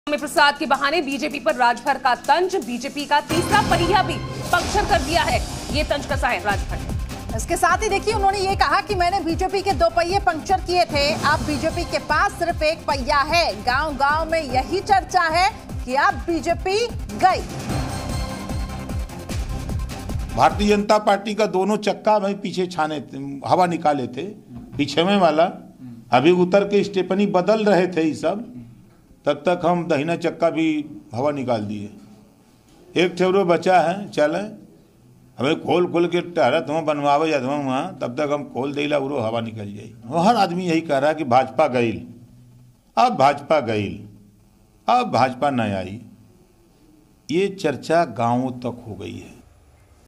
UP Election 2022 | स्वामी प्रसाद के बहाने BJP पर Om Prakash Rajbhar का तंज़ | CM Yogi
स्वामी प्रसाद के बहाने BJP पर राजभर का तंज़, कहा BJP का तीसरा पहिया भी पंक्चर. मैंने BJP के 2 पहिए पंक्चर किए थे.

About Channel